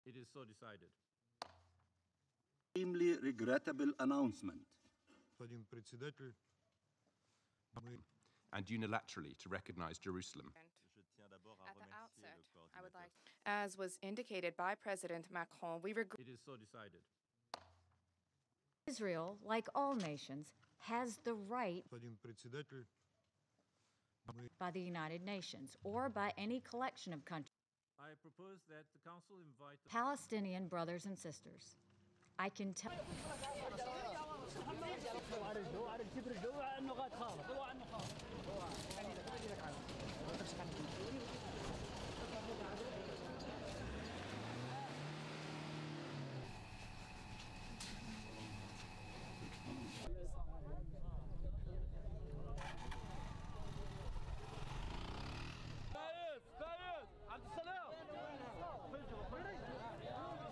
PeoplesVoice24.Tv
Two Palestinians shot dead and one critical in riots after Trump speech